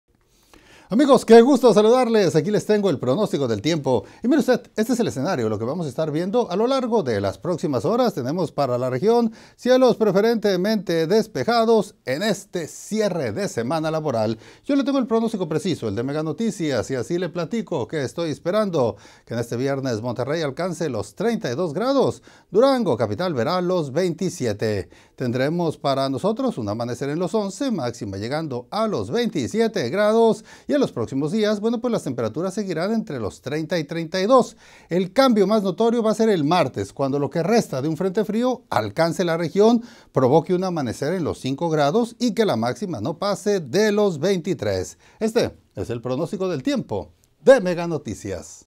Torreón: Pronóstico del clima para este jueves 21 de marzo de 2024
→ Entérate 🔵 🌡☀️ Conoce el Pronóstico del Clima 🌞🌦 en LaLaguna, para que tomes tus previsiones al respecto.